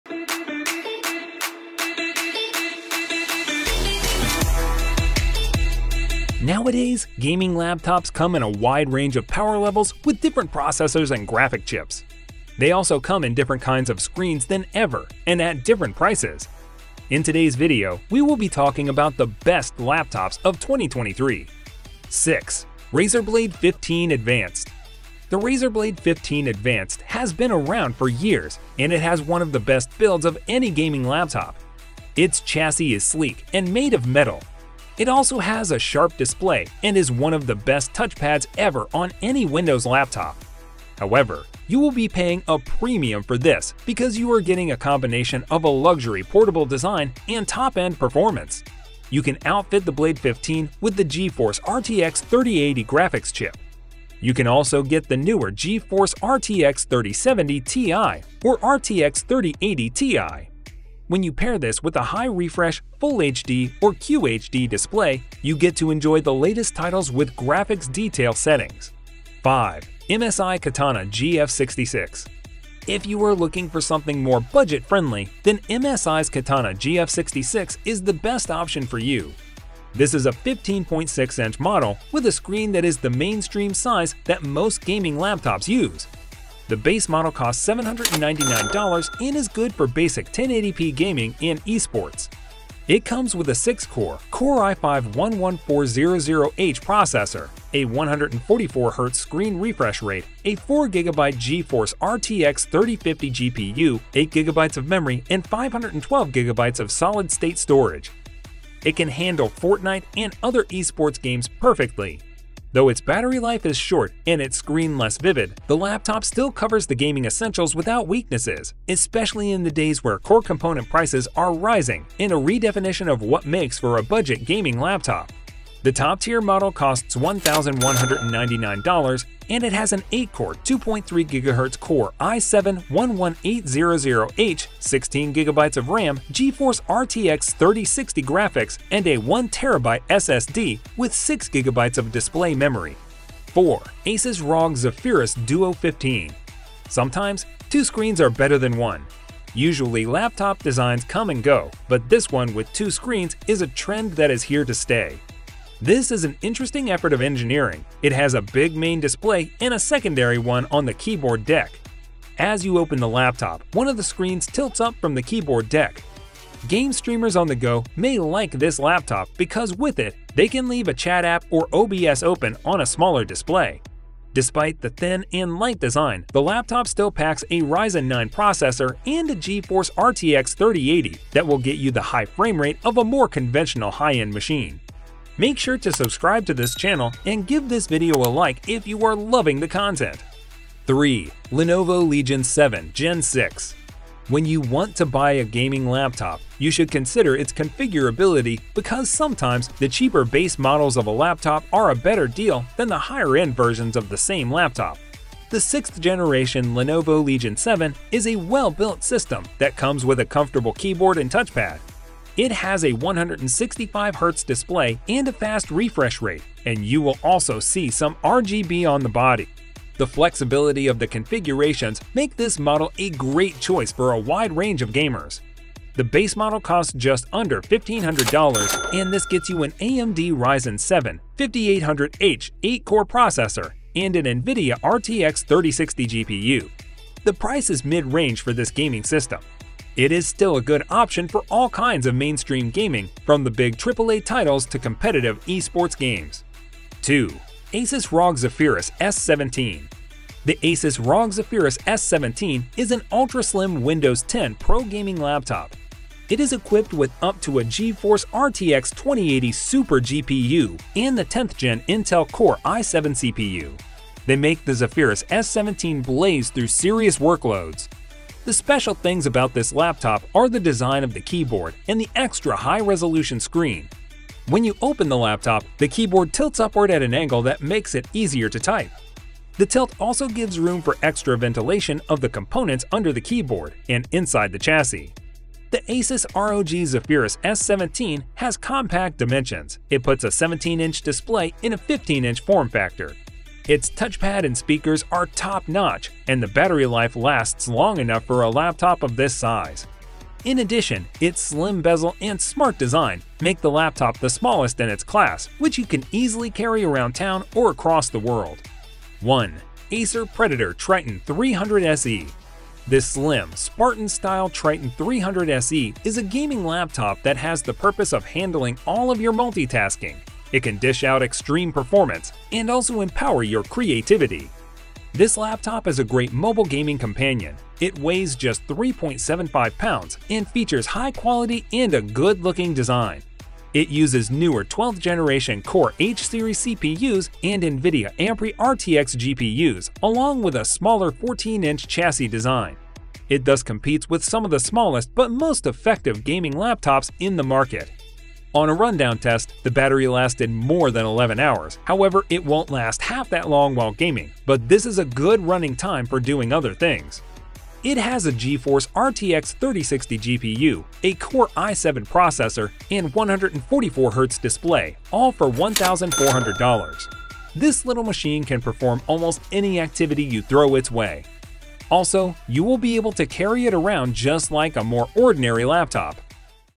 Best Gaming Laptops of 2023
Nowadays, gaming laptops come in a wide range of power levels with different processors and graphic chips. They also come in different kinds of screens than ever and at different prices. In today’s video, we will be talking about the best gaming laptops of 2023.

0:22 Razer Blade 15 Advanced
1:12 MSI Katana GF66
2:25 Asus ROG Zephyrus Duo 15
3:11 Lenovo Legion 7 Gen 6
4:07 Asus ROG Zephyrus S17
5:11 Acer Predator Triton 300 SE